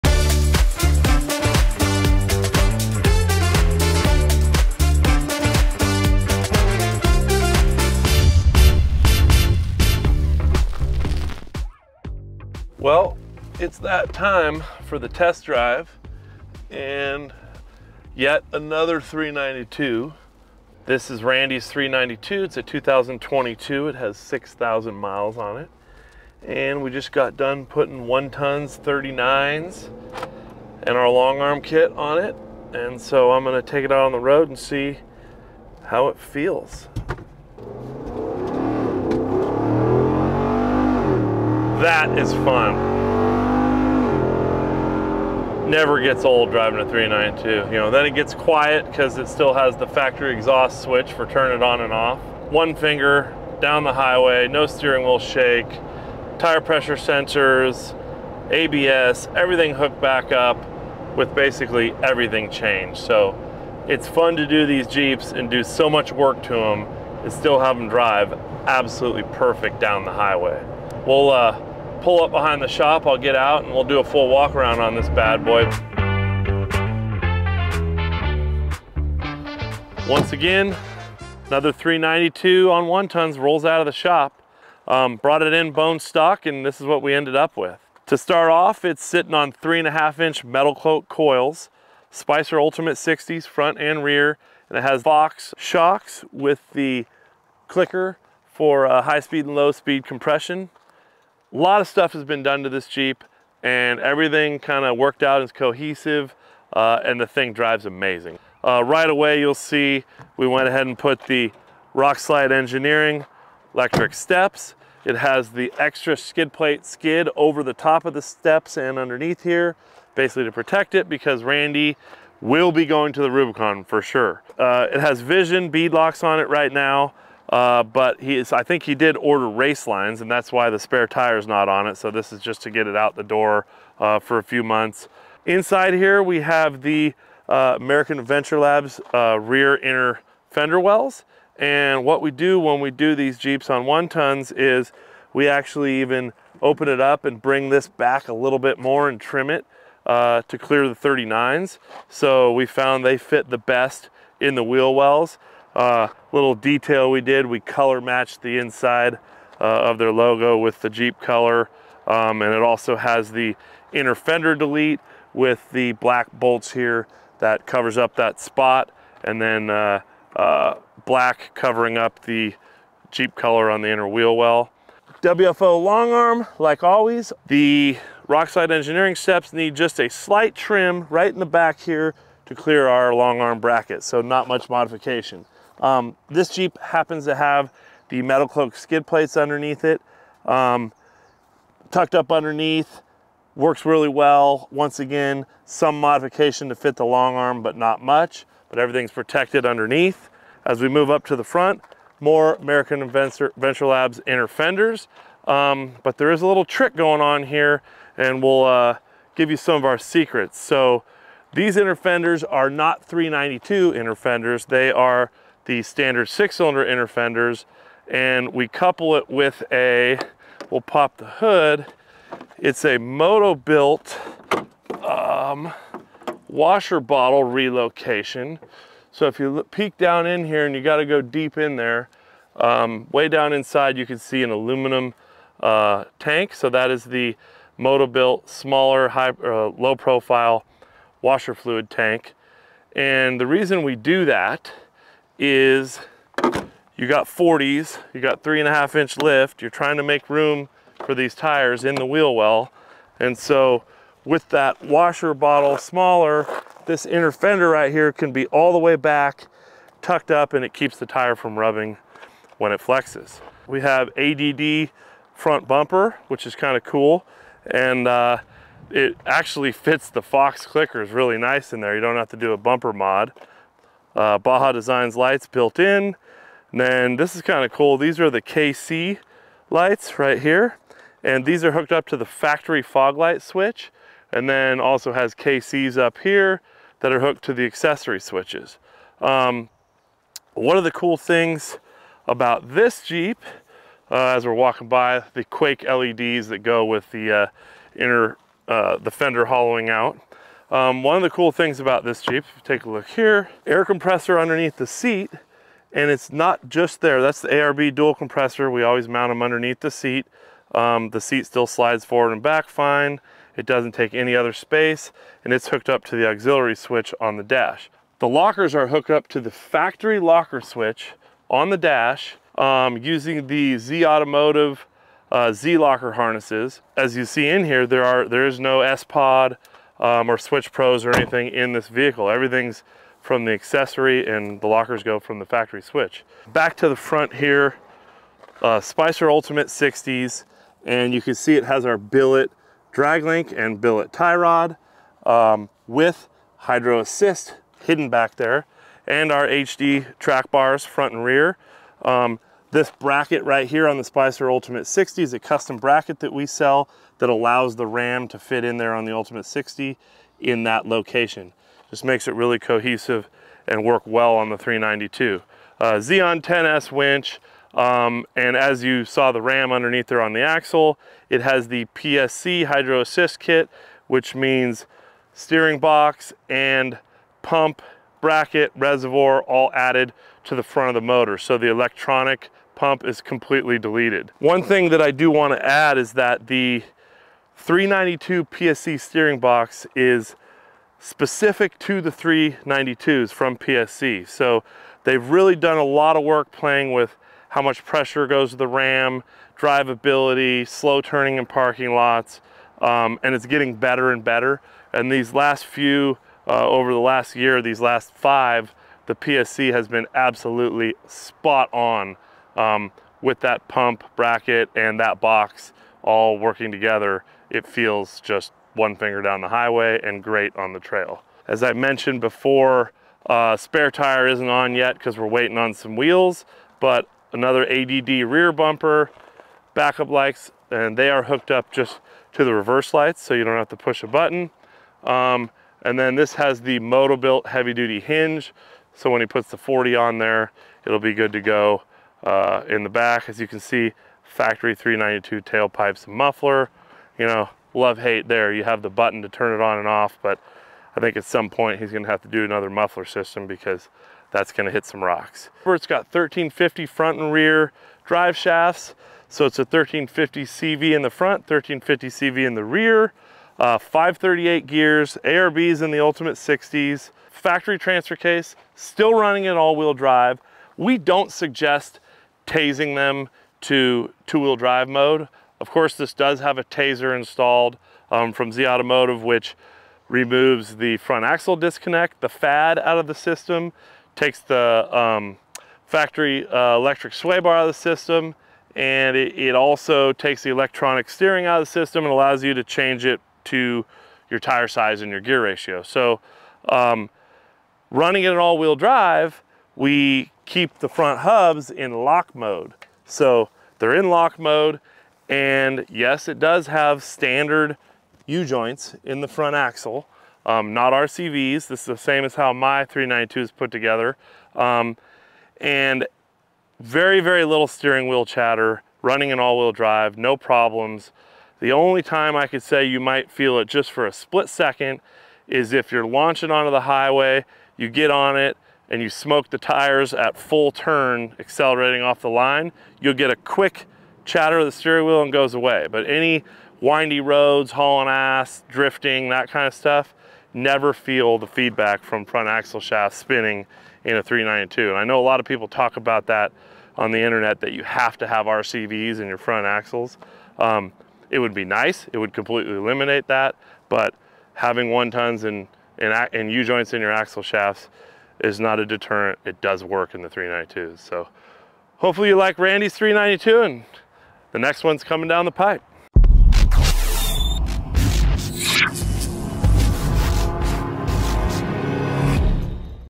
This Jeep Parties! | 392 Jeep Rubicon on 1-Tons
⚙️Offering you the best 4x4 Parts and Accessories

392 with a WFO Concepts Long Arm Kit and 1 tons? Sign us up!

WFO Parts Featured in this Video:

~WFO JEEP JL 4 LINK LONG ARM UPGRADE KIT

~WFO 2018+ JEEP JL/ 2019+ JEEP JT GLADIATOR - FRONT HD TRACK BAR